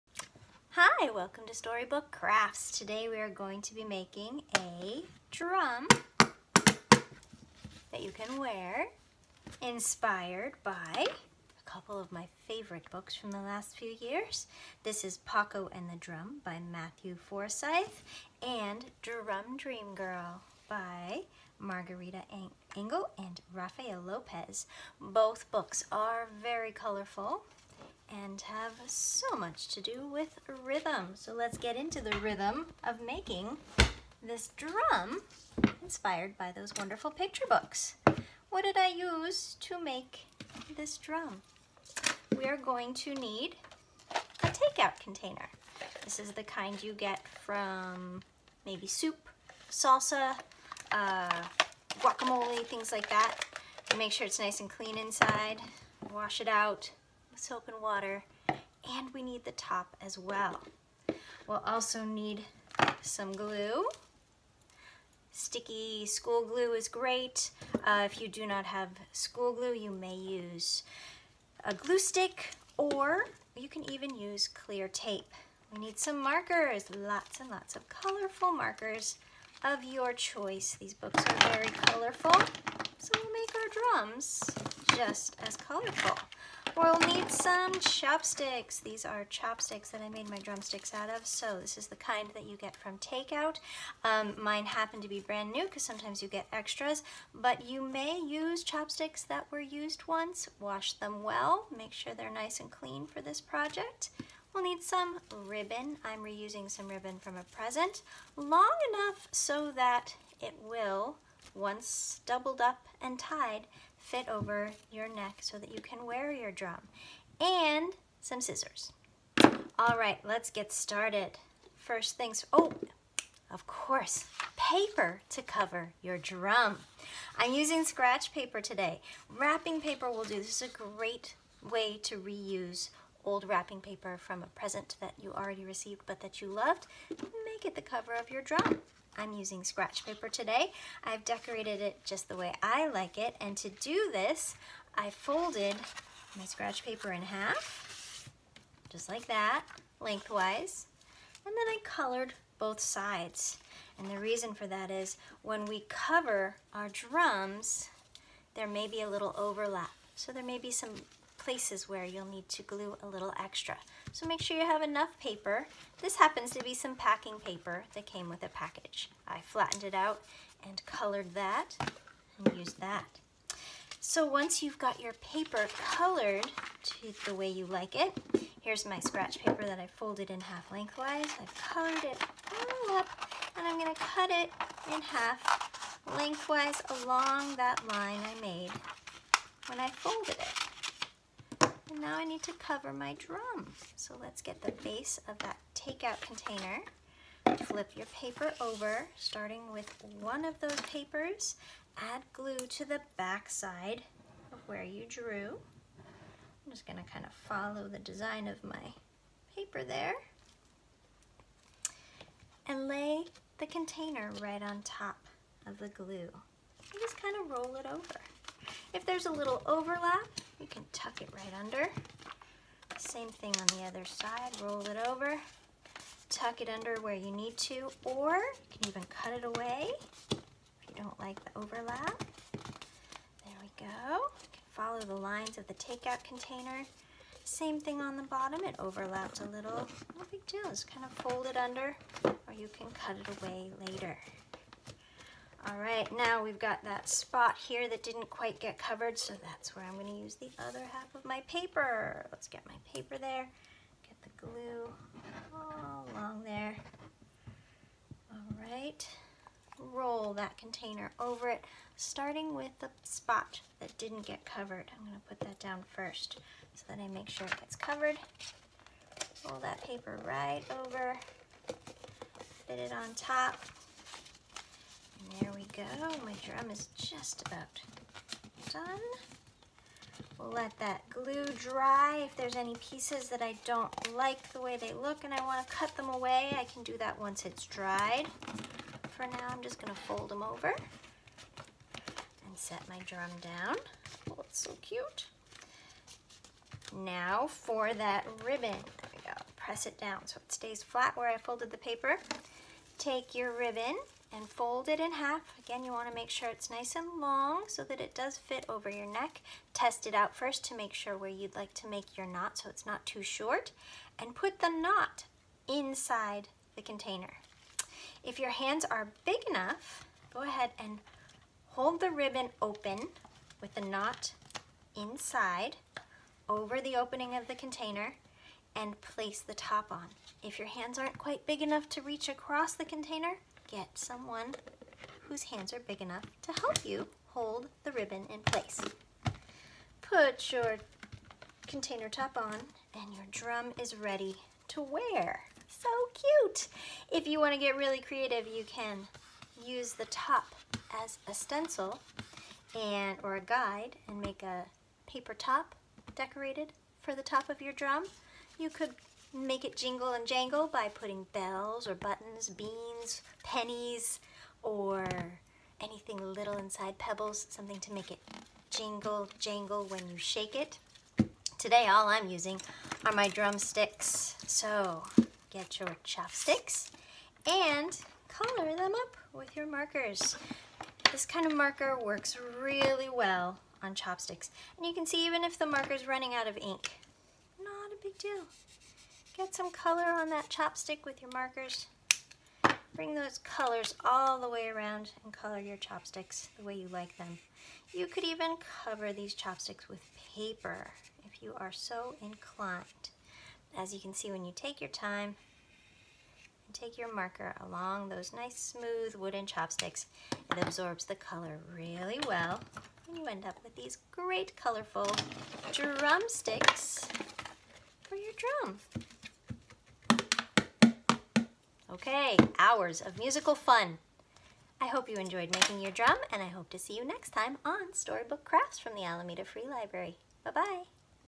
Alameda Free Library Storybook Crafts: Take-Away Drum
Make a drum you can take away with you using a take-away container inspired by Drum Dream Girl, by Margarita Engle, and Pokko and the Drum, by Matthew Forsythe, with Ms. Andie! You’ll need a clean, cylindrical plastic take-out container with a top, some clean wooden chopsticks, markers, scissors, scratch paper, a long string or ribbon, and glue. Drumroll please...